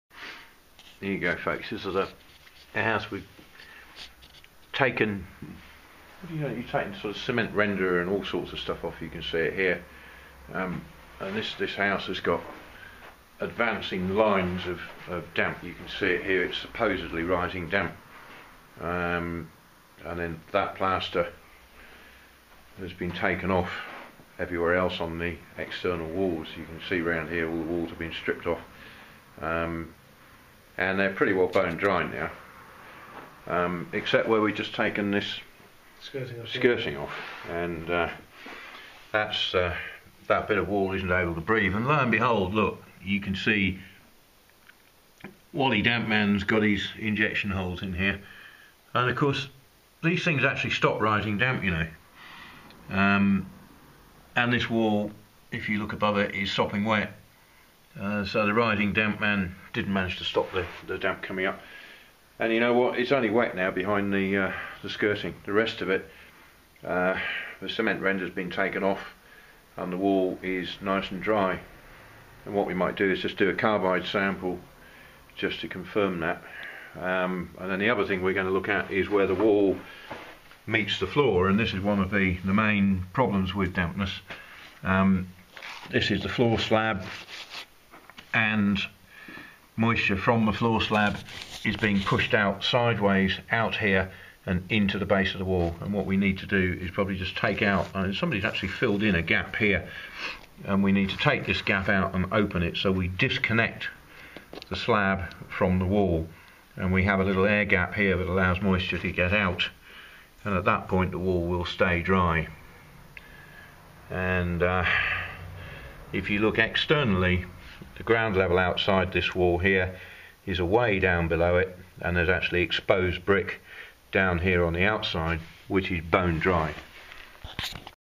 Rising damp? No! Its cement trapping moisture.
This wall was even injected.  The damp wallies had cement rendered it inside.  When taken off, the wall dried immediately. The skirting was still on when I arrived - taken of, it revealed wet bricks and injection holes.  Within hours the wall was drying out - it was sopping wet above the injection holes, showing that injection doesnt work.  The real reason for damp was bitumen covering clay floor tiles, which when removed, allowed the floor to dry.